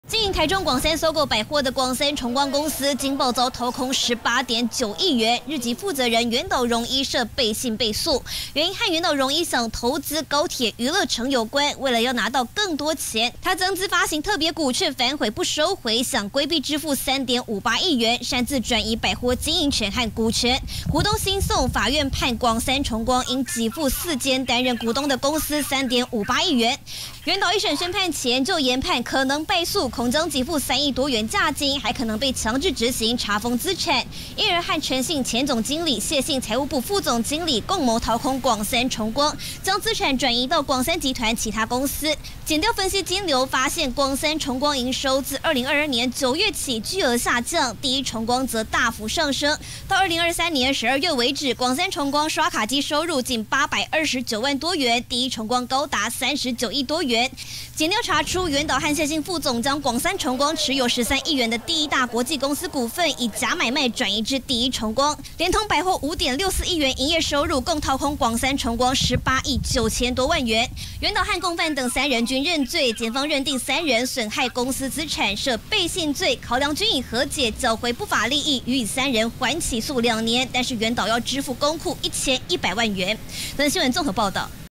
台中廣三SOGO爆掏空18.9億 日籍負責人涉背信被訴@newsebc
經營台中廣三SOGO的廣三崇光公司，驚爆遭掏空18.9億元，日籍負責人原島榮一為投資「高鐵娛樂城」，增資發行特別股卻反悔不收回，想規避支付3.58億元價金，還擅自轉移百貨經營權股權，涉及掏空遭檢方偵辦。

11/10~11/24 敬請鎖定51頻道！
⚾場次看這邊⚾
11/13 18:25 中華VS韓國
11/14 18:25 中華VS多明尼加